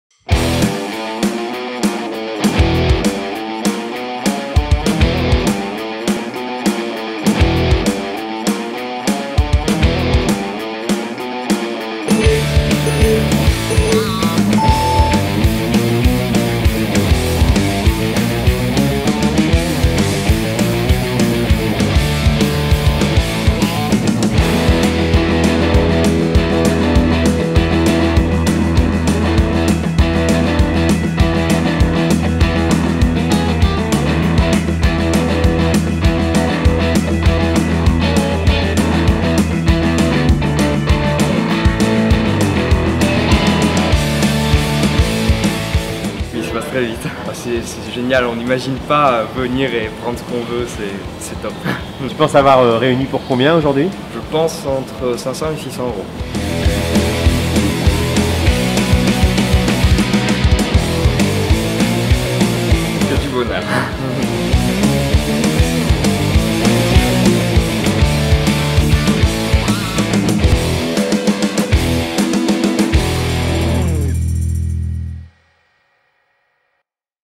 Hold Up Kaporal - Centre Valentine
Yoran, gagnant du concours "Hold Up Kaporal" a dévalisé le Kaporal store de La Valentine le mois dernier!
Serez-vous aussi rapides que lui?
Crédit vidéo: Marsatwork